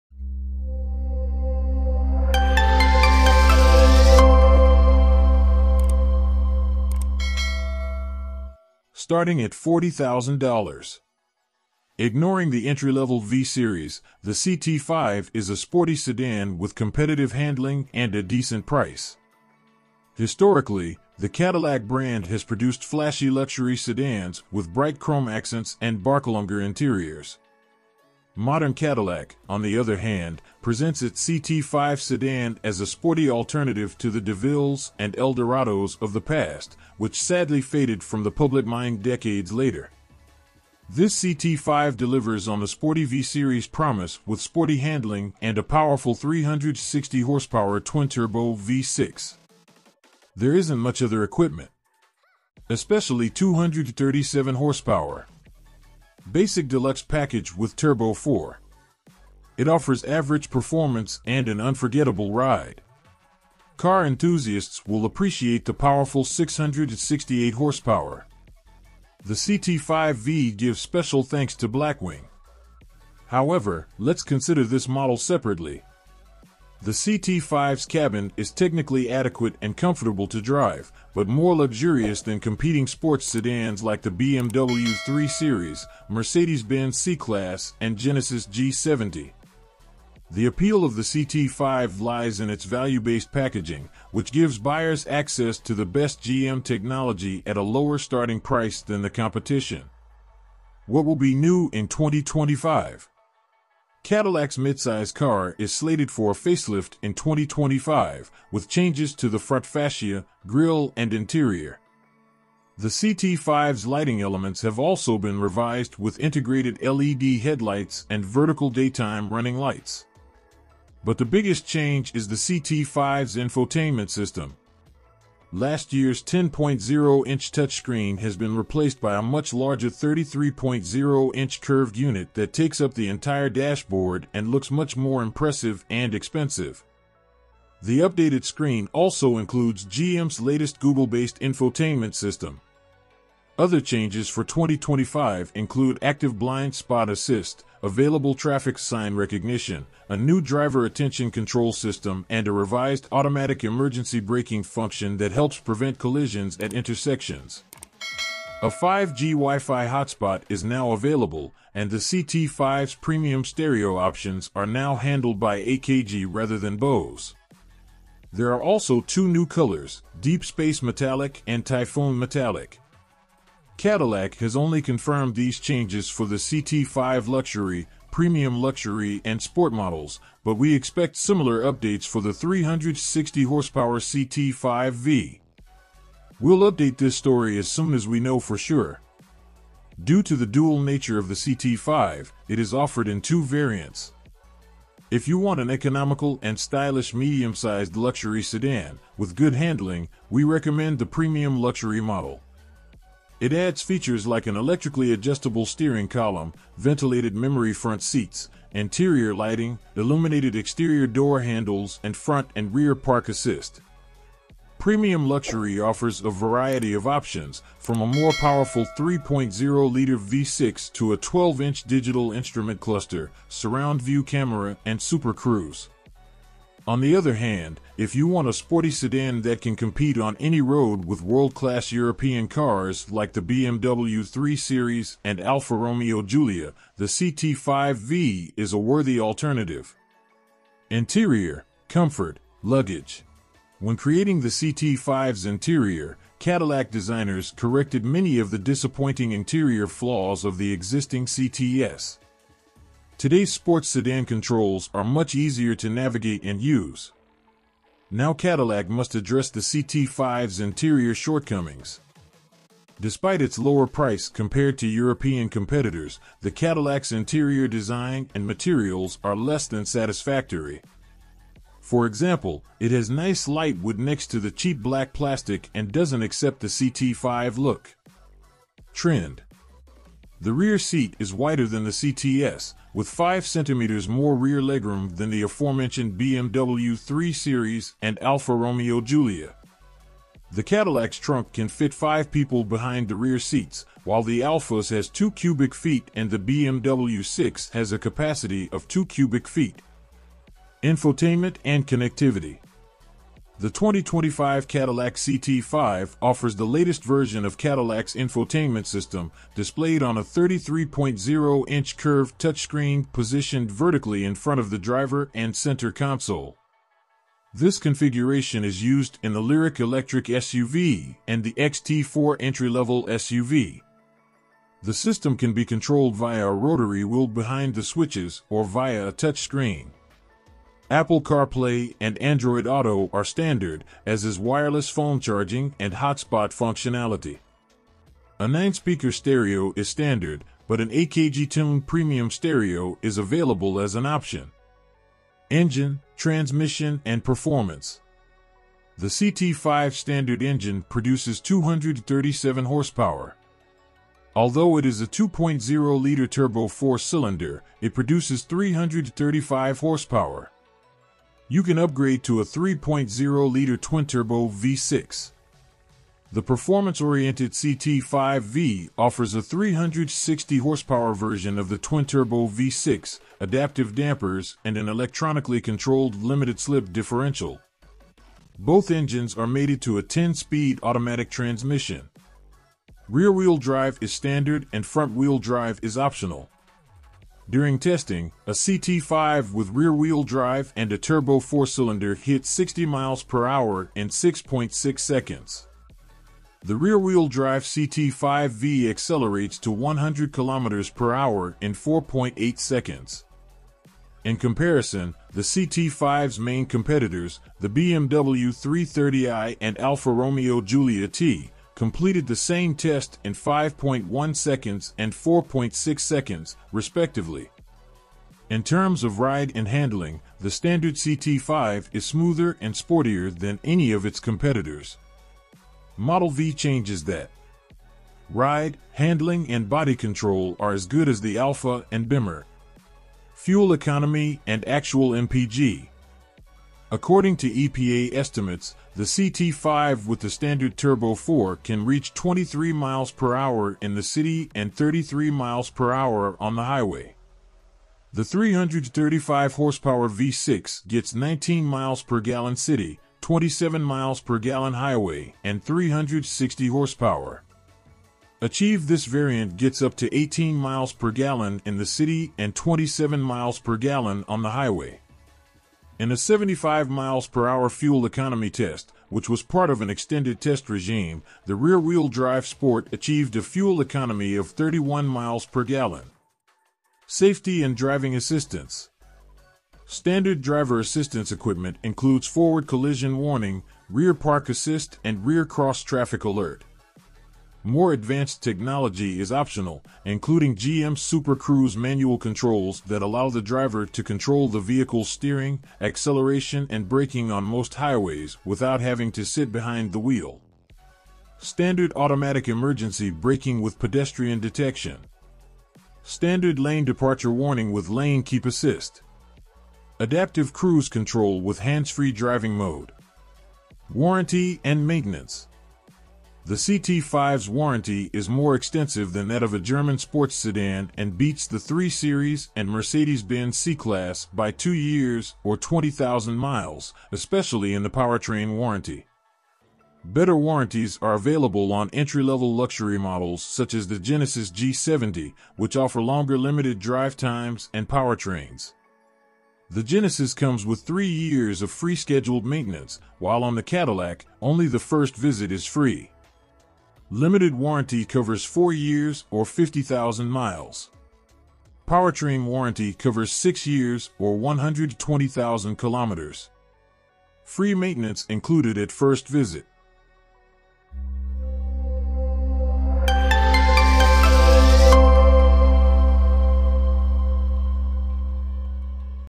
The All New 2025 Cadillac CT5 Official First Look.
2025 Cadillac CT5 Sedan Adds Sleek 33-Inch Screen, New Headlights